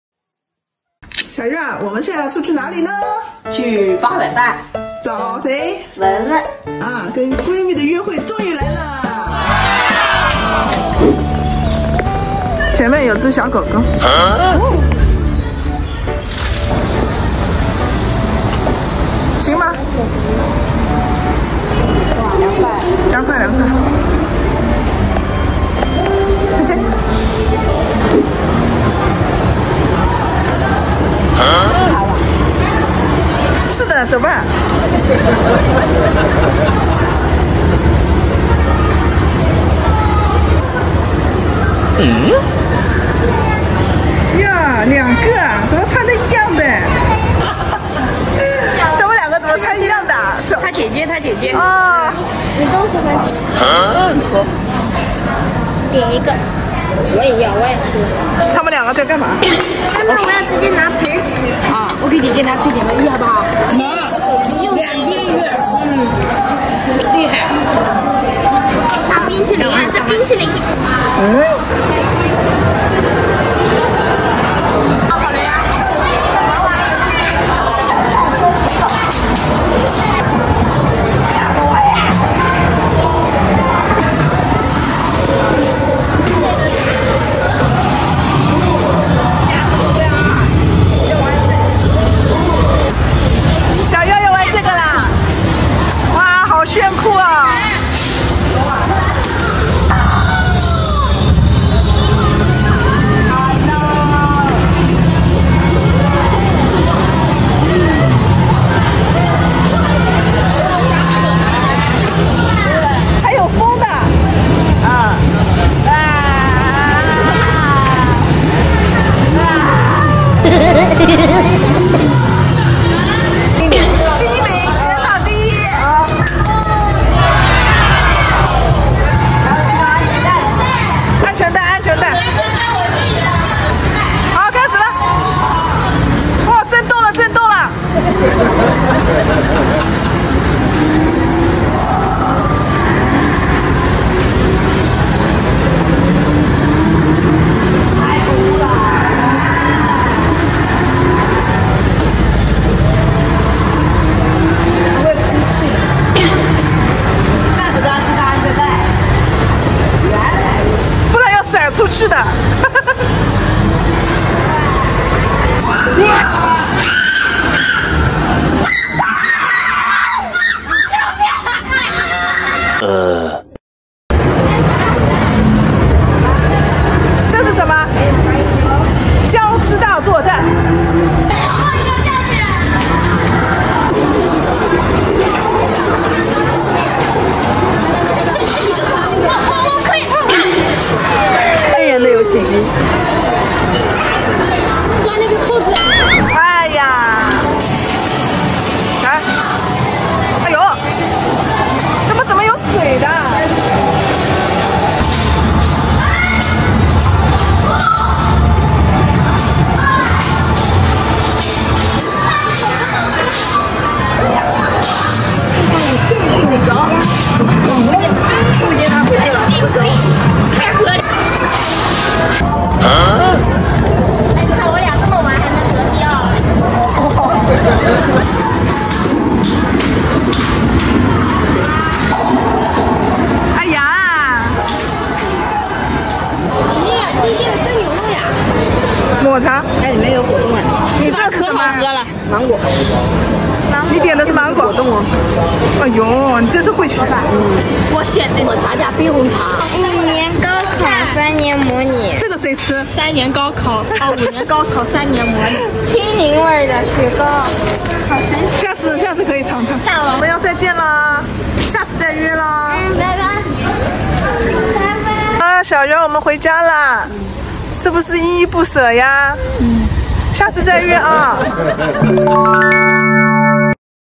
期末考结束，孩子约同学打电玩，没想到第一次玩赛车得了第一名？【叫我龙阿姨】
哈喽~大家好~这里是【叫我龙阿姨】在YouTube上的官方频道~
小鱼儿的妈，柴菜子的娘，一枚爱拍照爱拍视频的小阿姨